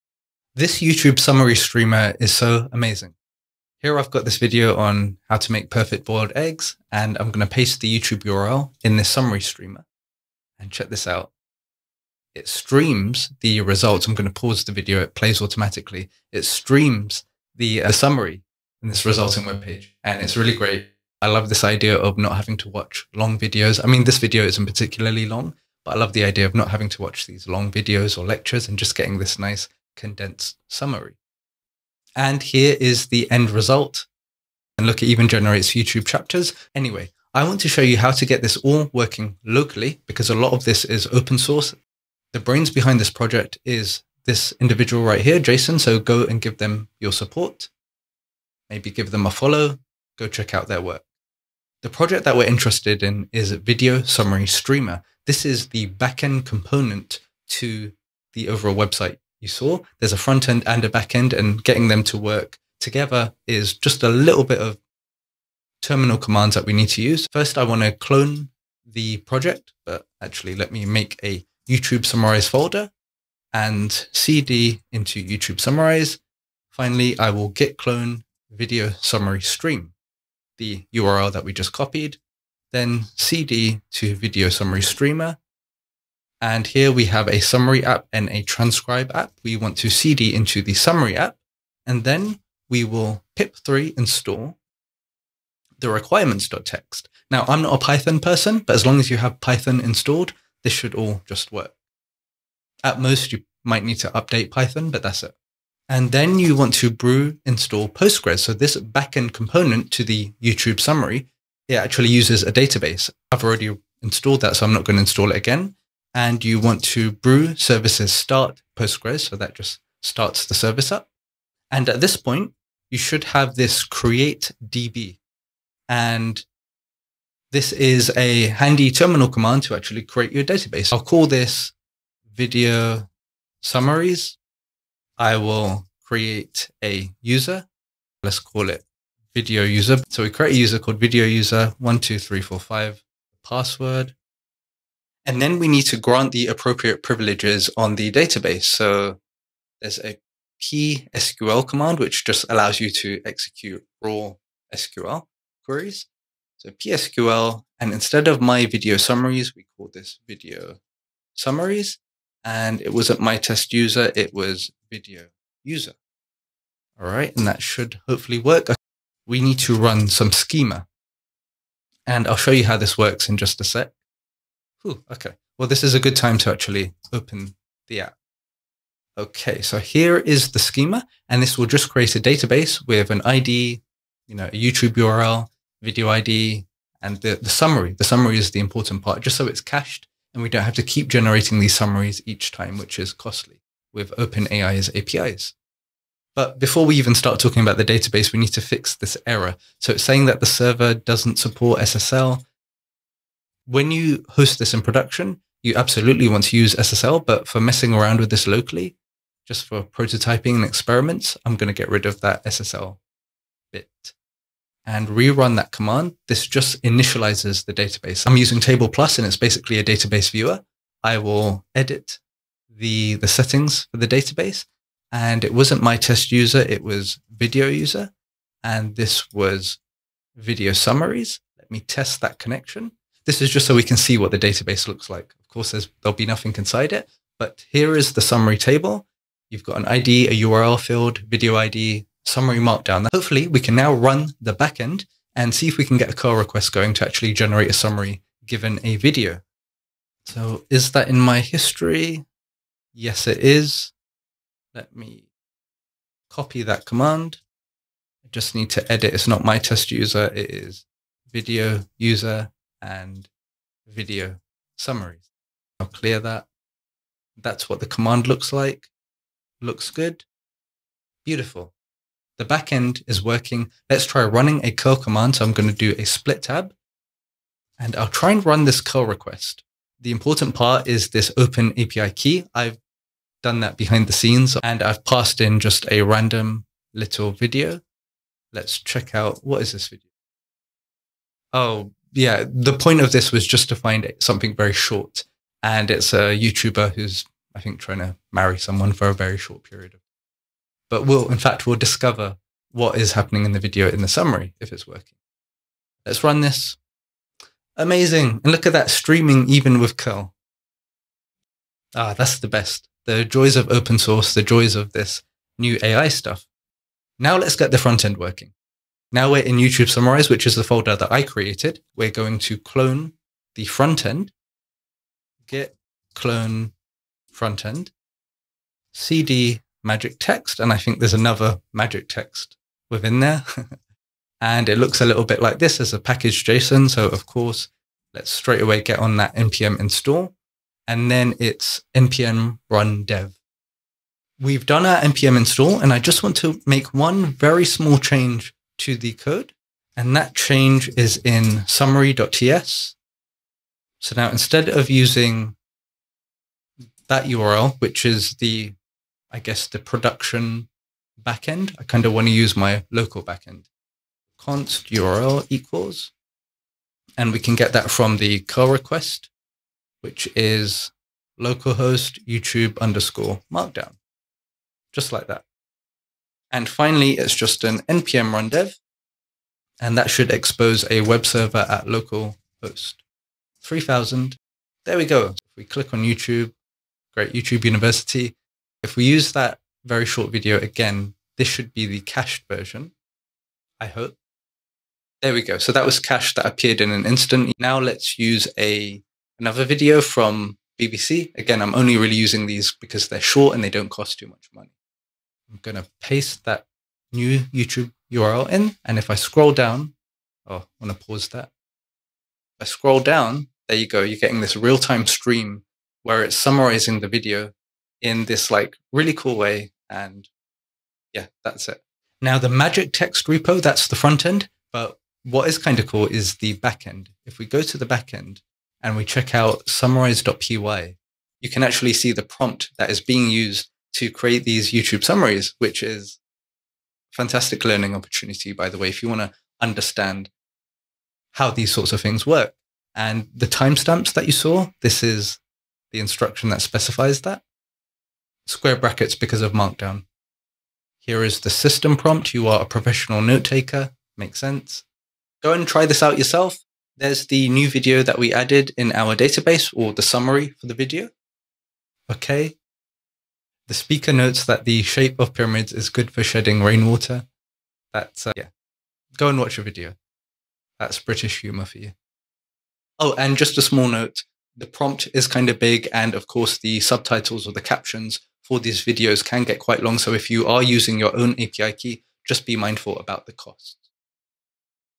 AI Video summaries: this will change how we consume videos!
If you want more tips like delivered straight to your inbox, make sure you sign up for the Dev Tips mailing list